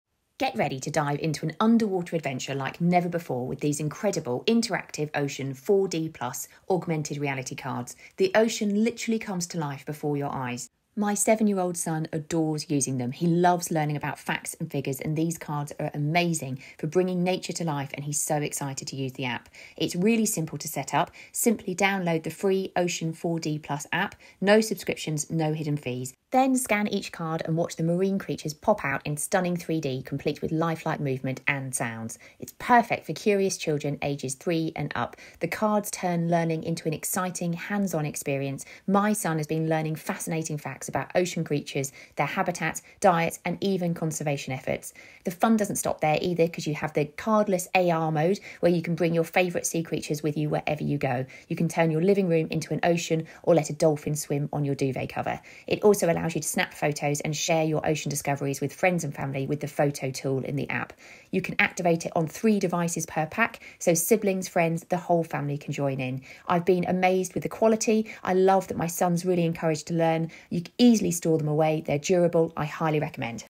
Ocean 4D+ Cards – Watch Marine Life Come Alive with Interactive AR Flashcards
Experience the vibrant underwater world with Ocean 4D+ Cards! Watch as marine life—sharks, dolphins, and colorful reef inhabitants—springs to life using the free Ocean 4D+ App on iOS and Android. Simply scan a card to unlock immersive 3D animations and explore detailed ocean zones that bring education and adventure together.

Perfect for families, educators, and ocean enthusiasts, Ocean 4D+ Cards transforms play into an unforgettable underwater journey. Dive in and start your adventure today!